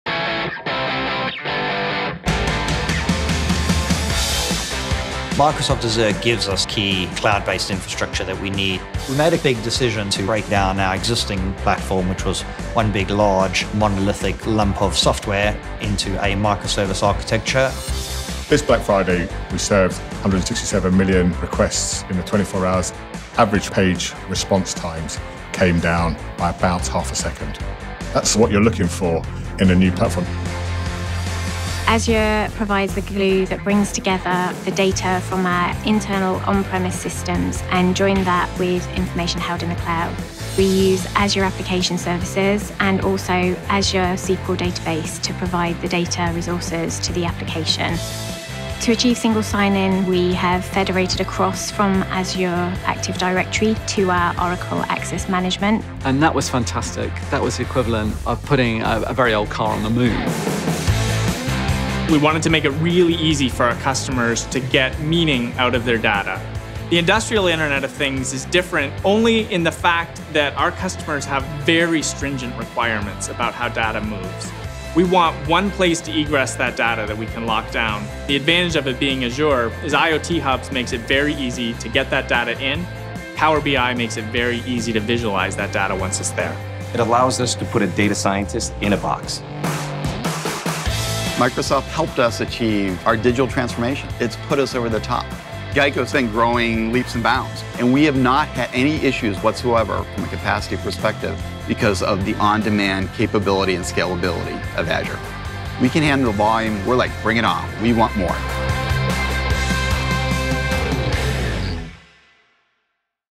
Microsoft Build 2017 | Azure Customers
Hear from Azure customers and how they use the different tools Azure provides to create business solutions. You will learn from customers across different industries such as ASOS, British Airways, Rockwell Automation, and GEICO.